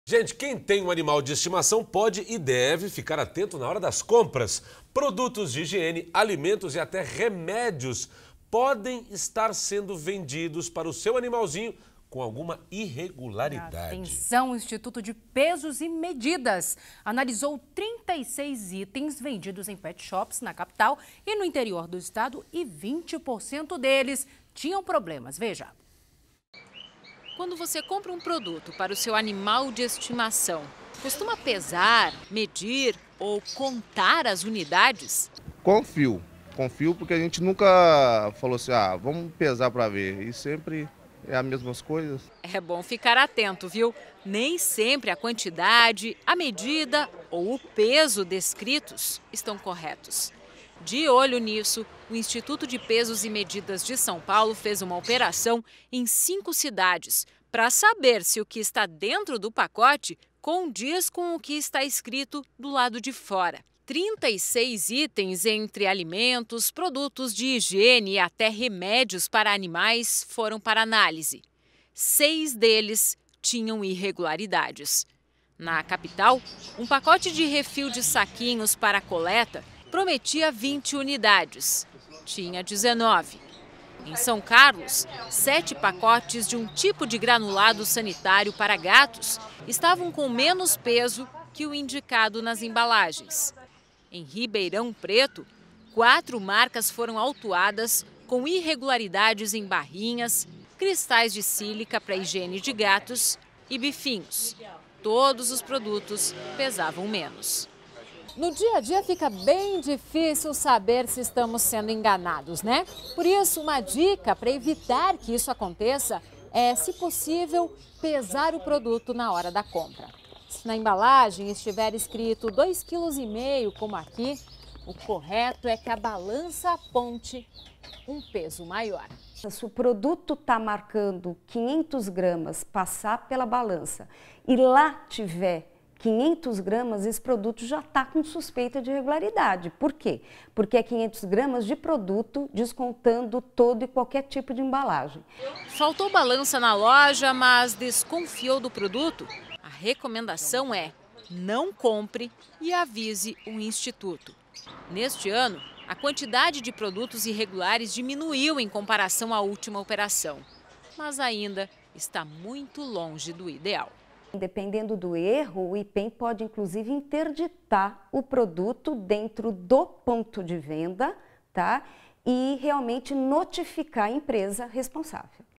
IPEM aponta irregularidades em pet shops de São Paulo
O Instituto de Pesos e Medidas de São Paulo encontrou irregularidades nos produtos vendidos por pet shops da capital e do interior do estado. Para não ser enganado, é recomendável que o cliente pese o produto, sempre que possível, na hora da compra. Para assistir ao conteúdo na íntegra, acesse PlayPlus.com